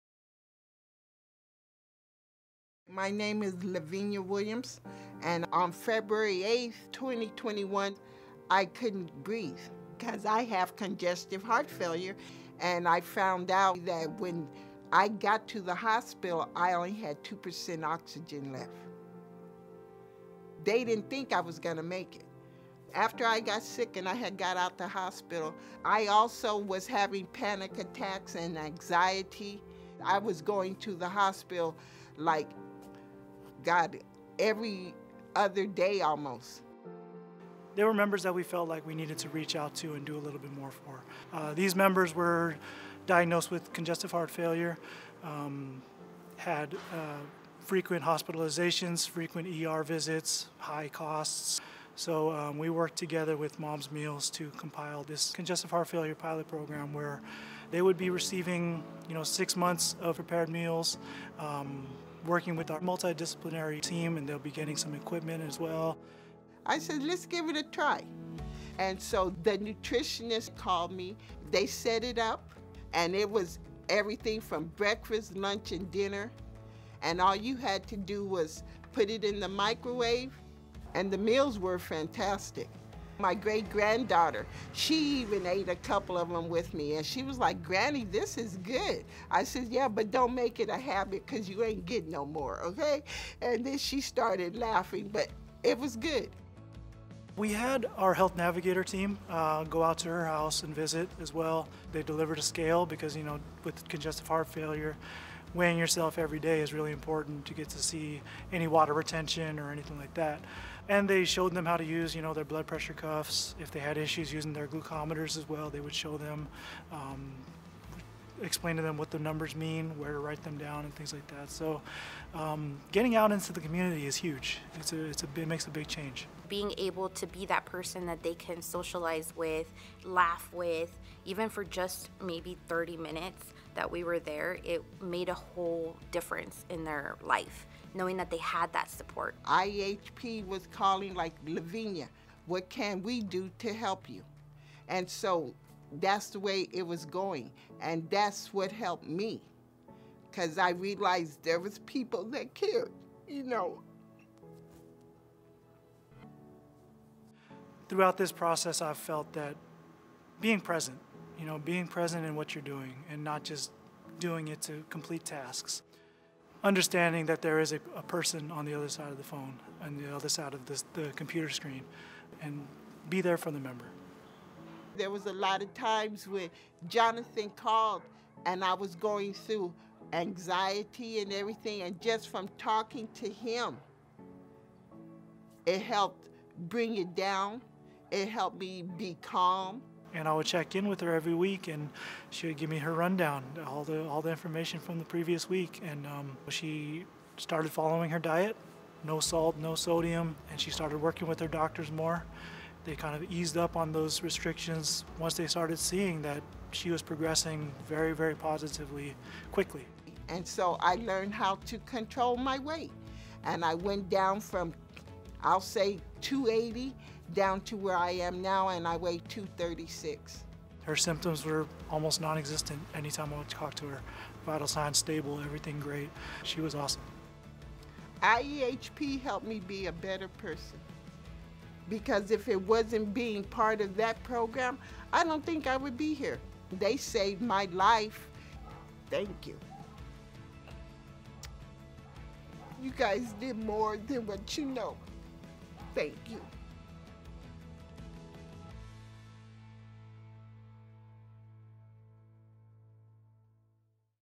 Congestive Heart Failure Pilot - IEHP Member Testimonial
A pilot program by Inland Empire Health Plan (IEHP) and Mom's Meals helped people with congestive heart failure improve their health and reduce their medical costs.

The program provided participants with fully prepared lower-sodium meals, fresh produce and dry goods; nutrition education; a digital scale; and home visits. The results were impressive: participants saw a 50% reduction in both trips to the emergency room and hospitalizations, and their cost to care fell from $7.2 million to $5.4 million annually.

In this video, you'll hear from IEHP member Lavinia Williams, who credits the program with saving her life. She'll share how the program helped her to eat healthier, lose weight, and manage her condition.

If you're interested in learning more about how medically tailored meals and other health-related tools can help people with congestive heart failure, watch this video.